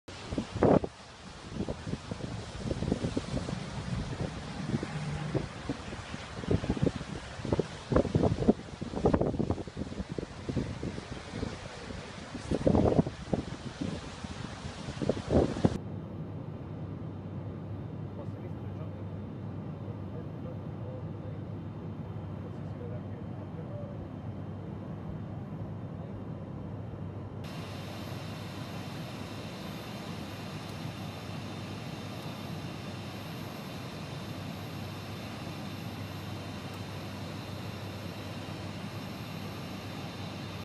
Nave si incaglia sotto costa nello Stretto di Messina
La nave mercantile “Gustav Maersk” in navigazione da Rijeka e diretta a Gioia Tauro, durante le manovre per lo sbarco del pilota a seguito del transito nello Stretto di Messina da sud a nord, per cause in corso di accertamento, non è riuscita a riprendere la rotta, incagliandosi in località Torre Cavallo (RC). La nave è stata disincagliata dai rimorchiatori.